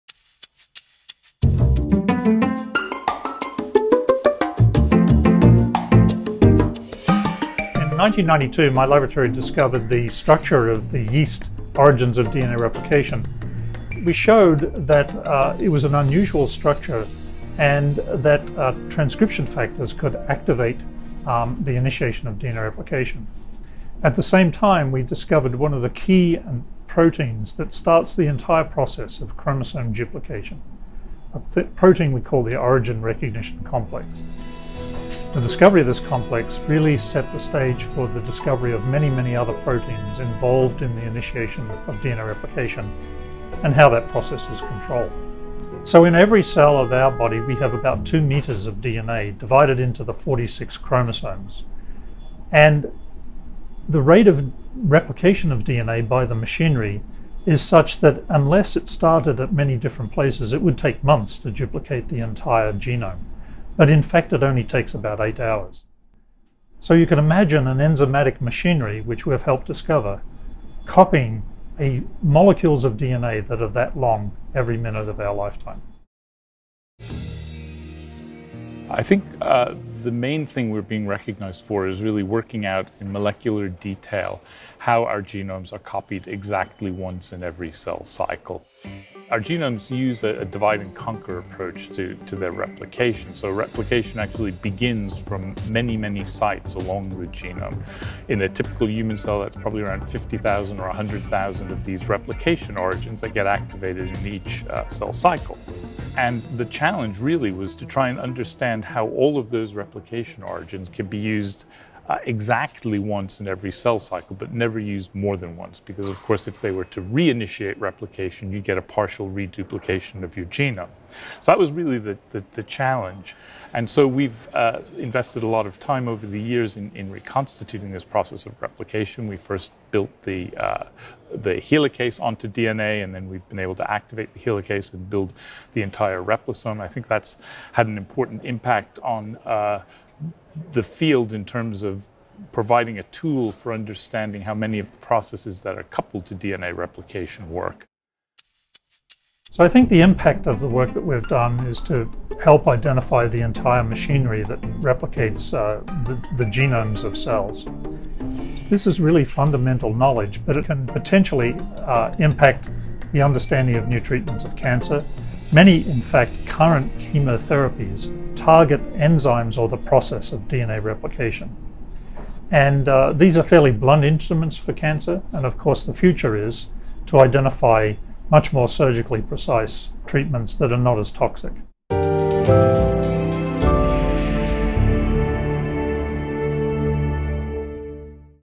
2019 International Award: Bruce Stillman & John Diffley
Dr. Bruce Stillman, Ph.D., FRS
President, Cold Spring Harbor Laboratory, Cold Spring Harbor, New York

Dr. John F.X. Diffley, Ph.D., FRS FMedSci
Associate Research Director, The Francis Crick Institute, London, U.K.

Awarded “For their pioneering research on the eukaryotic DNA replication cycles including initiation, regulation and responses to DNA damage”